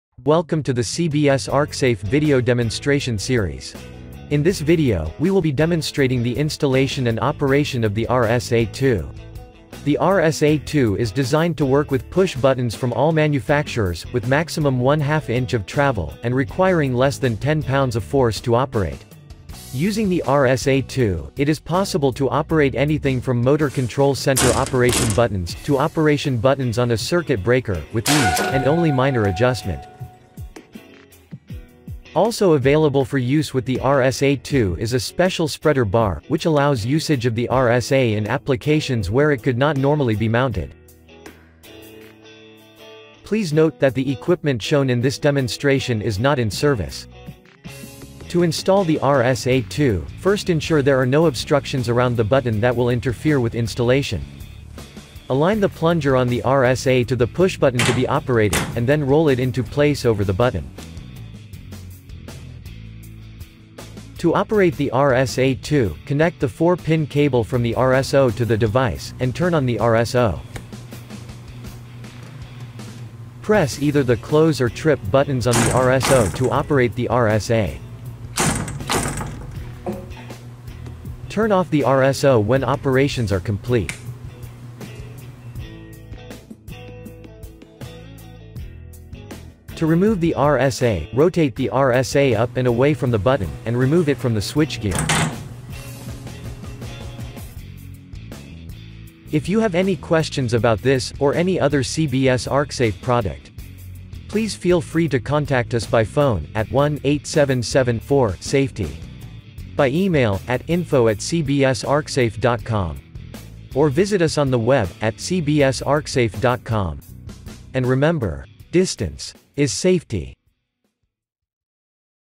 CBS ArcSafe® RSA-2 Single Application Remote Switching Tool Operation Demonstration
CBS ArcSafe remote switch actuators are designed to remotely charge, close, and/or trip various styles and types of electrical equipment.

Any remote switch actuator can be custom-built for any electrical device you have available.

The remote switch actuators are most commonly assisted by strong magnets so that modifications are not required to your switchgear. This also allows operators to easily move the remote switch actuator from one breaker to another.

Each CBS ArcSafe remote switch actuator is custom built to your electrical equipment so that it can safely operate your electrical equipment outside the arc-flash boundary.

There are numerous CBS ArcSafe remote switch actuators that enable the system to be used with circuit breakers, motor controls, and other electrical equipment that use a variety of:

- Operating pushbuttons, such as circuit breaker trip and close buttons
- Operating levers, such as those found on motor control center buckets
- Operating lever or rotary style charging handles requiring single or multiple strokes
- Operating rotary and pistol grip switches, such as breaker control switches
- Engaging or disengaging mechanical interlocks, such as circuit breaker interlock foot-pedals or pushbuttons